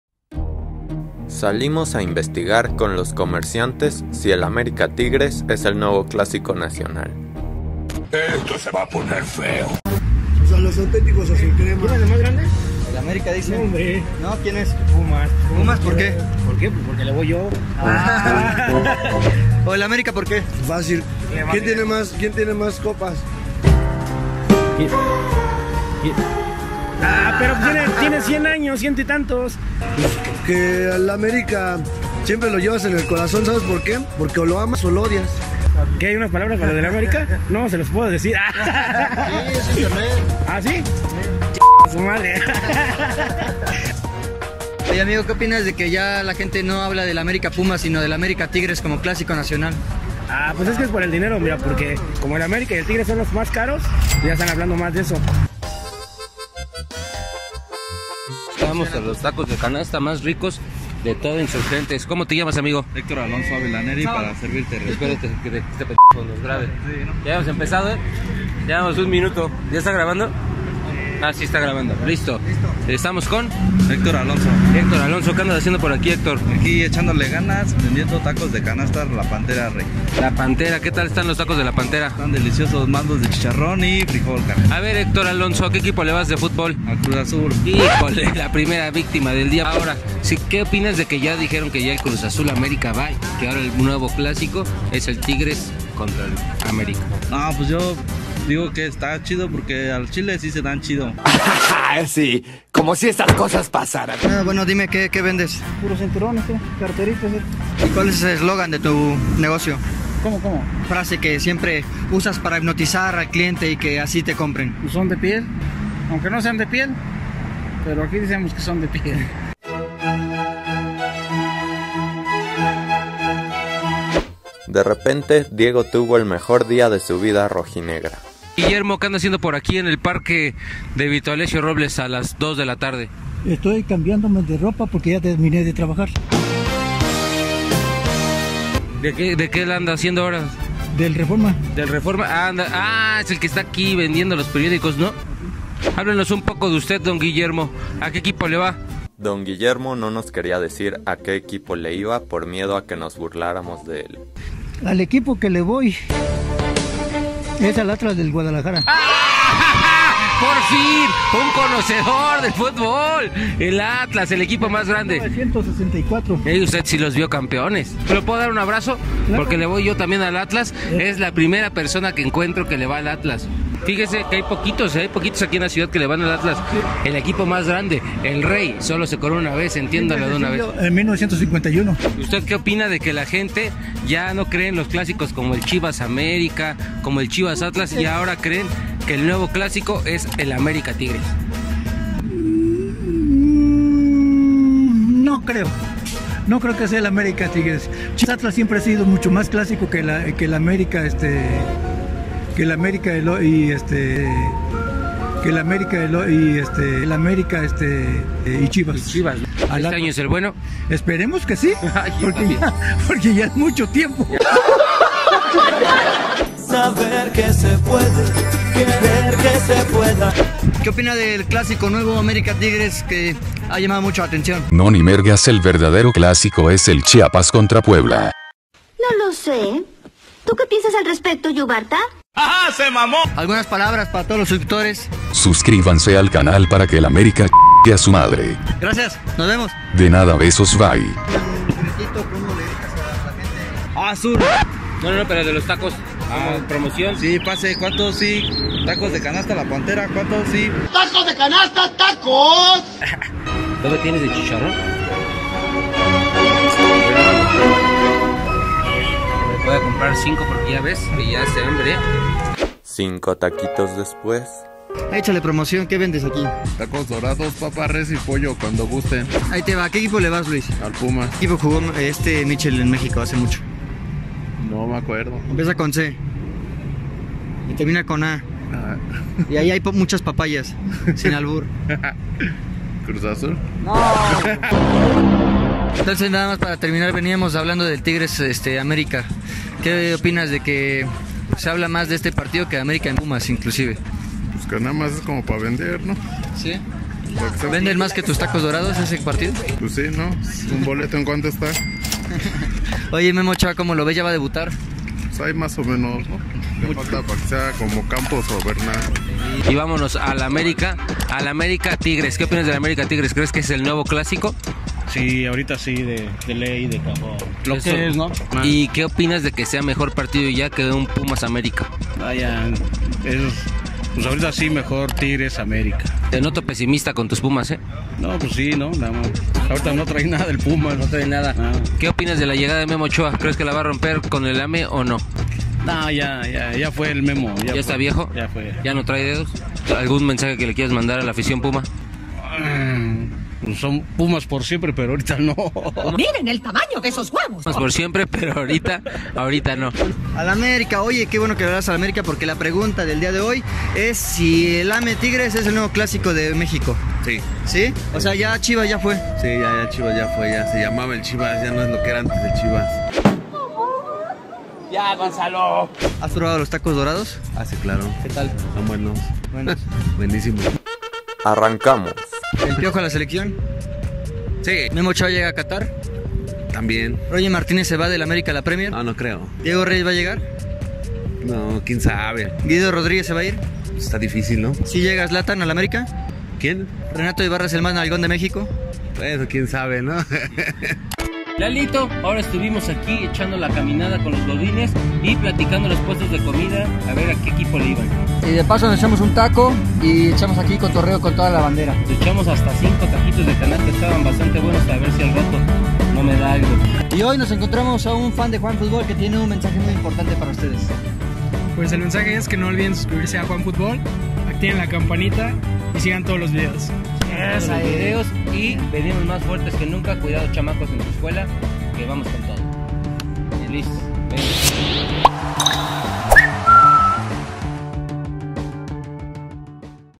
AMÉRICA VS TIGRES NUEVO CLÁSICO NACIONAL | SONDEOS JUANFUTBOL
La gente opinó sobre el posible nuevo clásico de México: AMÉRICA VS TIGRES. Respuestas inesperadas.

¿Quieres más del mejor storytelling deportivo?

Ve nuestros videos ▼